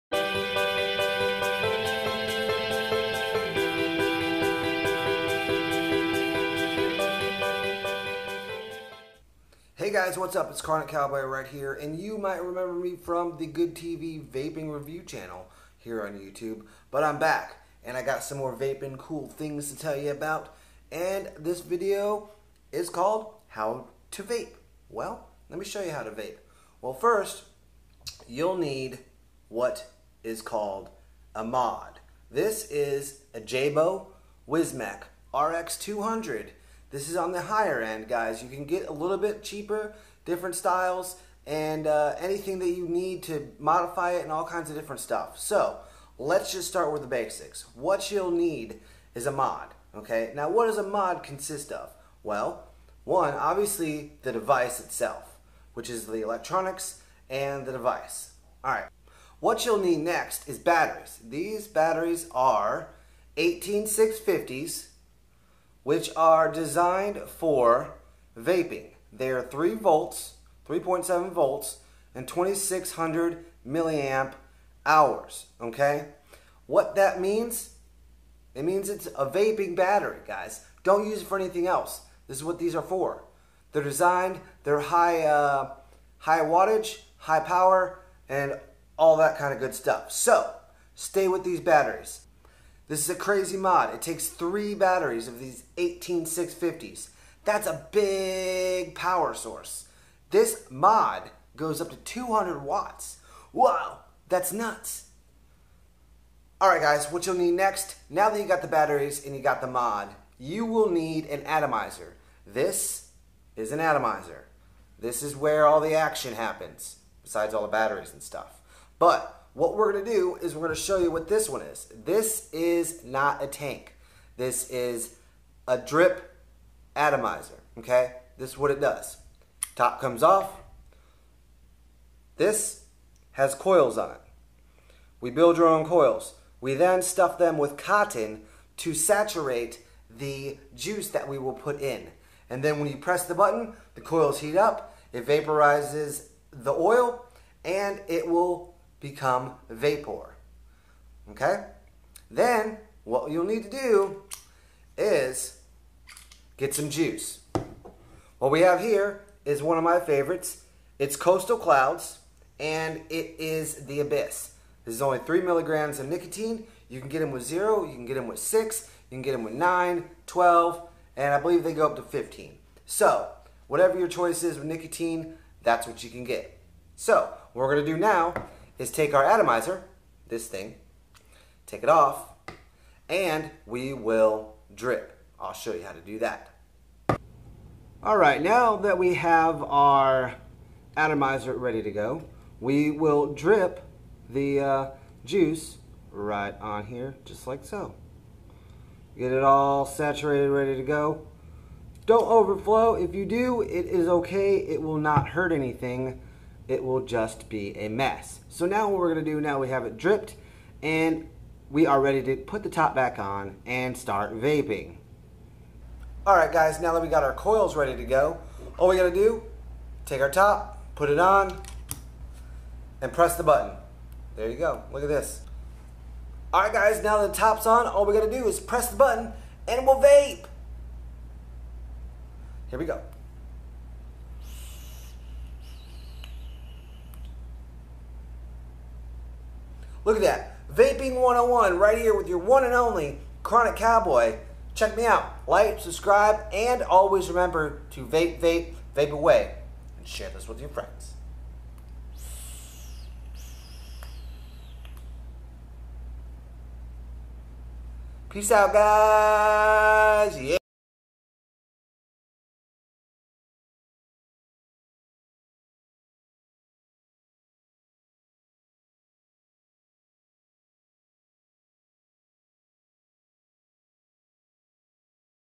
How To Vape 101
In this short educational video Chronic Cowboy teaches you the basics of vaping and how to vape. Thanks-CC.